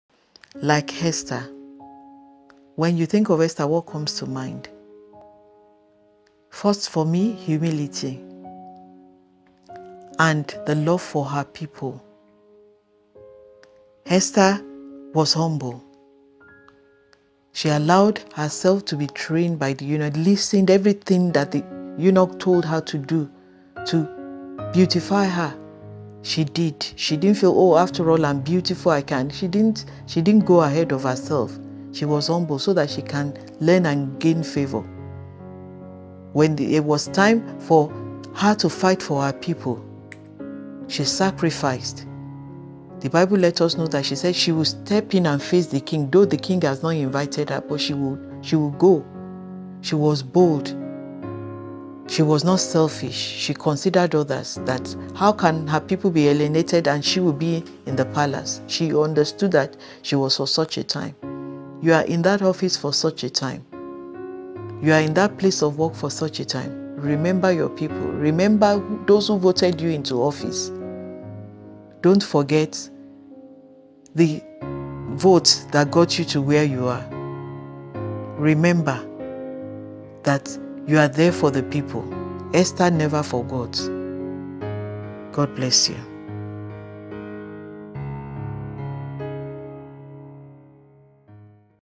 Like Esther | Daily Motivation | Pastor Bim
Watch this insightful motivation teaching by Pastor Bim
When you think of Esther what comes to mind?

Humility

Love for her people

Esther did not go ahead of herself in order to learn and gain favour

Esther was bold , she was not selfish, she remembered and stood for her  people when she was in the palace

You’re in your office for a purpose don’t forget the vote that got you your position in office.

Esther never forgot

Read Esther 2 vs 2 to 20 to know the behaviour of Esther as a slave woman before she became Queen.

Esther 2 vs 15
Now when the turn of Esther, the daughter of Abihail the uncle of Mordecai, who had taken her for his daughter, was come to go into the king , she required nothing but what Hegai the king's chamberlain, the keeper of the women, appointed. And Esther obtained  favour in the sight of all them that looked at her. (King James Version).

Esther 2 vs 17
And the king loved Esther above all the women, and she obtained grace and favour in his sight more than all the virgins; so that he set the royal crown upon her head, and made her queen instead of Vashti.(King James Version).

Read Esther 5 vs 1 to 17 to understand how queen Esther fasted and prayed to God for her people the Jews when they were about to be destroyed.(King James Version).

Esther 5 vs 6
So Hatach went forth to Mordecai unto the street of the city, which was before the king’s gate.(King James Version).

Esther 5 vs 7
And Mordecai told him of all that had happened unto him, and of the sum of the money that Haman had promised to pay to the king’s treasuries for the Jews, to destroy them.(King James Version).

Esther 5 vs 8
Also he gave him the copy of the writing of the decree that was given at Shushan to destroy them, to shew it unto Esther, and to declare it unto her, and to charge her that she should go in unto the king, to make supplication unto him, and to make request before him for her people. (King James Version).

Esther 5 vs 16
Go, gather together all the Jews that are present in Shushan, and fast ye for me, and neither eat nor drink three days, night or day: I also and my maidens will fast likewise; and so will I go in unto the king, which is not according to the law: and if I perish, I perish. (King James Version).

Esther 5 vs 17
So Mordecai went his way, and did according to all that Esther had commanded him. (King James Version).

The king’s heart is in the hand of the LORD, as the rivers of water: he turneth it whithersoever he will.(King James Version).

Humility pays Esther was chosen by God to be a Queen after been a slave because of her Humility.

FACEBOOK: Pastor Bim TV(PLEASE ADD)